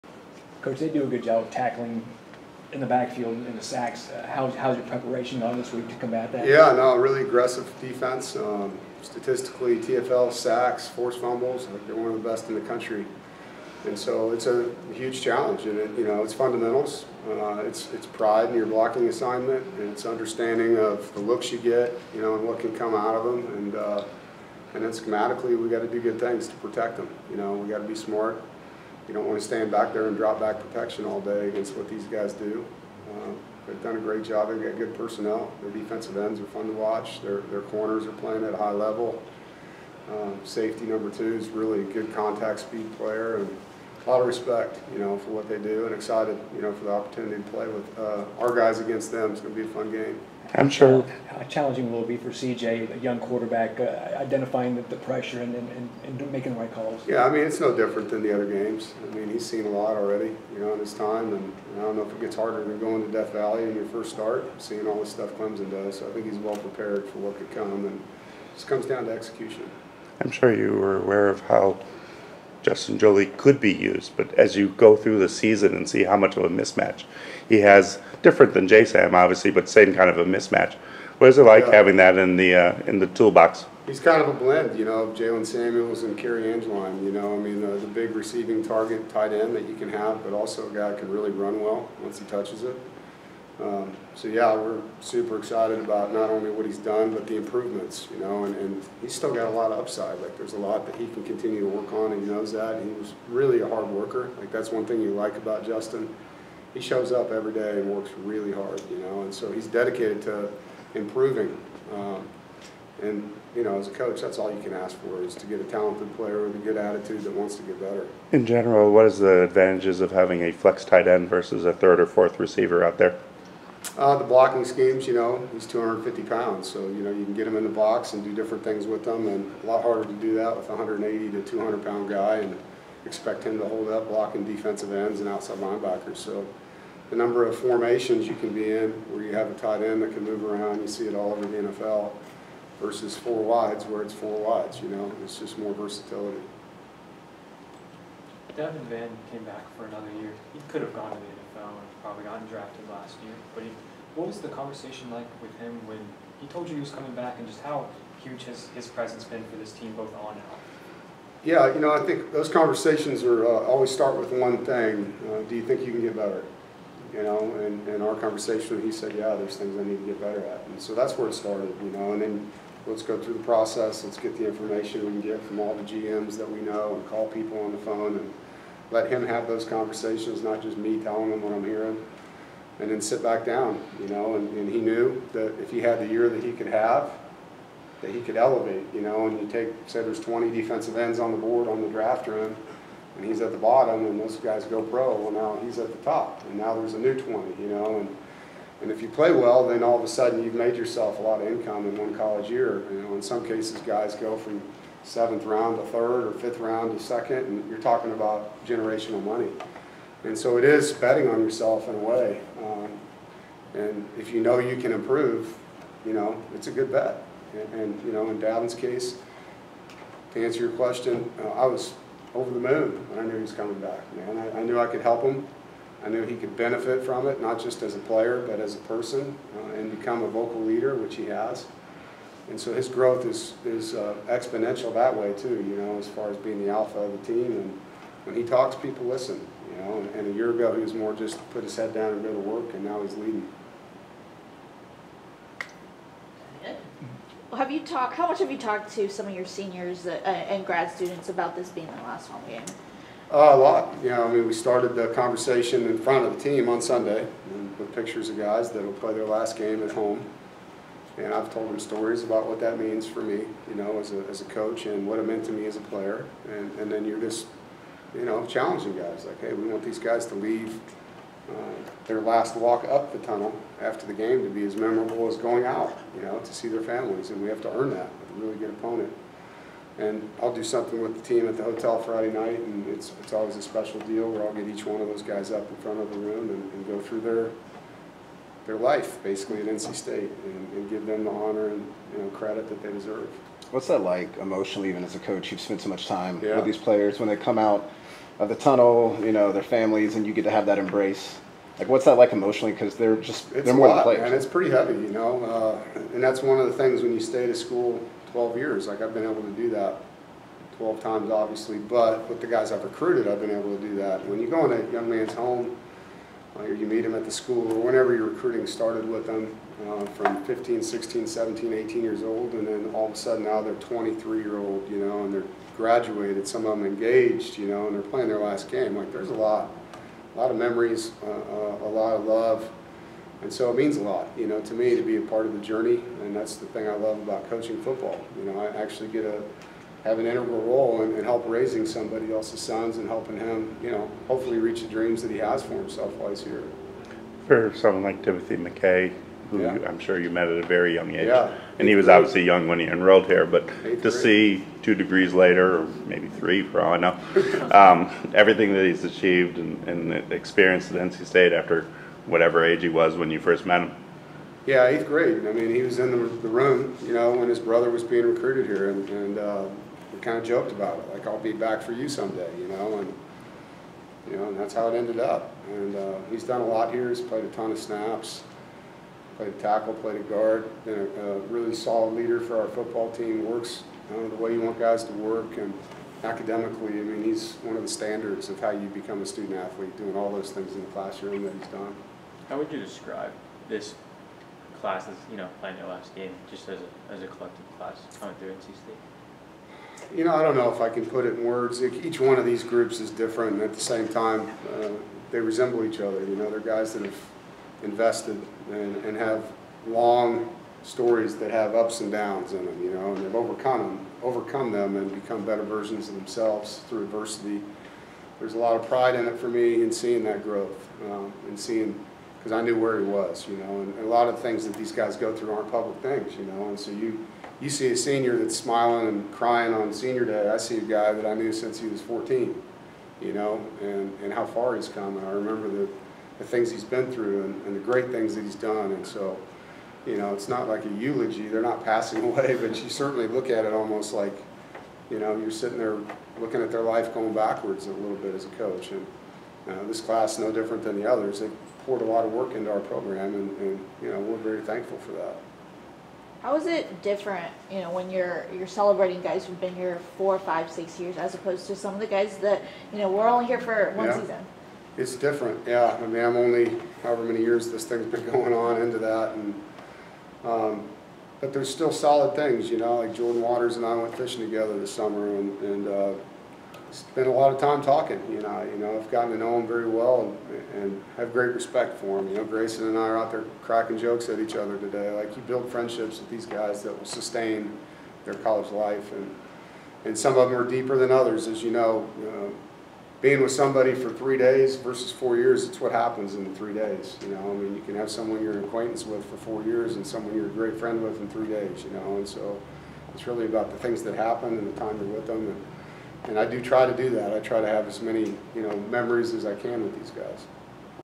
NC State coach Dave Doeren before Duke game
NC State coach Dave Doeren took questions from the media Nov. 7, 2024, two days before hosting Stanford.